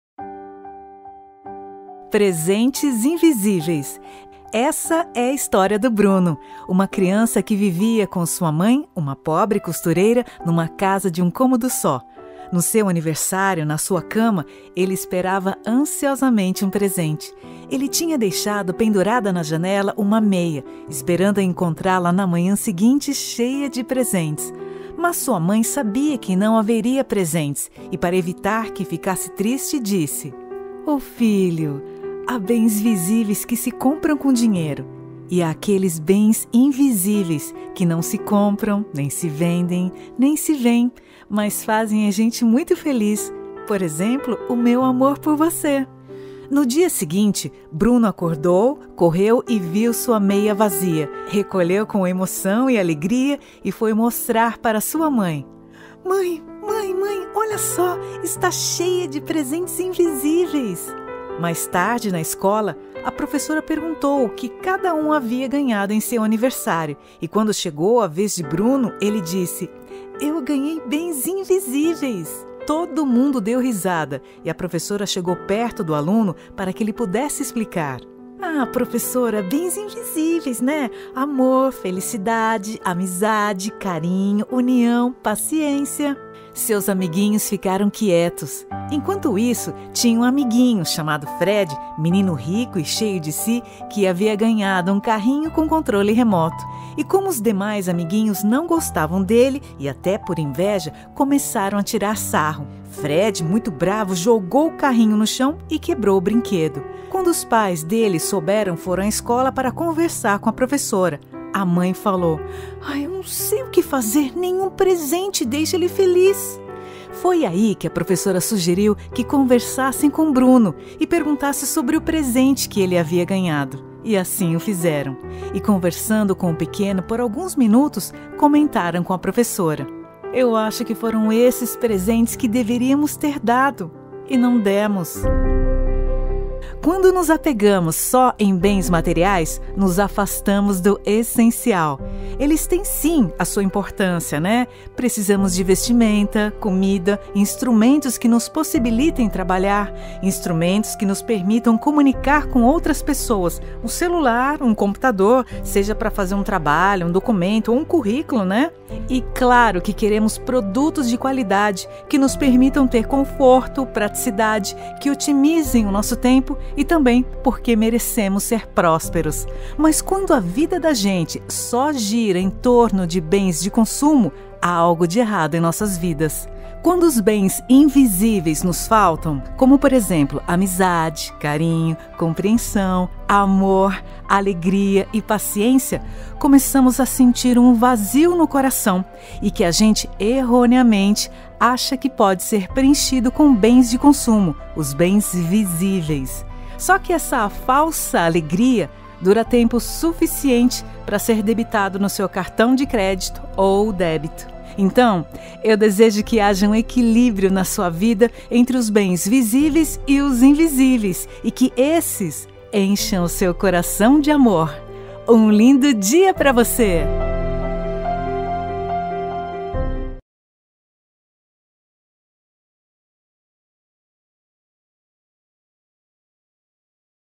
"Presente invisíveis" contada por Mayumi Sam
Essa história nos fala sobre o apego excessivo aos bens materiais. Há os bens invisíveis que são, por exemplo, a amizade,amor, carinho, paciência, harmonia... E o importante é que haja um equilíbrio entre os bens visíveis e invisíveis e que eles encham o seu coração de amor.

Narração e produção (áudio/vídeo): Mayumi Sam
Veiculação: em 23/09/2021 no Bom Dia Nossa Rádio - 94.1 _______________________________________________________

Presentes invisíveis

Essa é a história do Bruno, uma criança que vivia com sua mãe, uma pobre costureira, numa casa de um cômodo só. No seu aniversário, na sua cama, ele esperava ansiosamente um presente. Ele tinha deixado pendurada na janela uma meia, esperando encontra-la na manhã seguinte, cheia de presentes.

Mas sua mãe sabia que não haveria presentes e, para evitar que ficasse triste, disse:
“Filho, há bens visíveis que se compram com dinheiro e os bens invisíveis, que não se compram, nem se vendem, nem se vêem, mas fazem a gente muito feliz: por exemplo, meu amor por você”

No dia seguinte, o filho acordou, correu e viu sua meia vazia. Recolheu com emoção e alegria e foi mostrar pra sua mãe.

“Mãe, olha só, está cheia de bens invisíveis”

Mais tarde, na escola, a professora perguntou o que cada um havia ganhado em seu aniversário e quando chegou a vez de Bruno, ele disse:

“Eu ganhei bens invisíveis”

Todo mundo deu risada e a professora chegou perto do aluno para que ele pudesse explicar.

“Ah professora, bens invisíveis: amor, felicidade, amizade, carinho, harmonia, união, paciência...”

Seus amiguinhos ficaram quietos

Enquanto isso, tinha um amiguinho Fred, menino rico e cheio de si que havia ganhado um carrinho com controle remoto. E como os demais colegas não gostavam dele, e até por inveja, começaram a tirar sarro. Fred, bravo jogou o carrinho e quebrou o brinquedo. Quando os pais dele souberam foram à escola para conversar com a professora. A mãe falou:

- Não sei o que fazer, nenhum presente o deixa feliz.

Foi aí que a professora sugeriu que conversasse com Bruno e perguntasse a ele sobre o presente que havia ganhado e assim o fizeram. E conversando com o pequeno por alguns minutos, comentaram com a professora:

- Acho que foram esses presentes que deveríamos ter dado e não demos.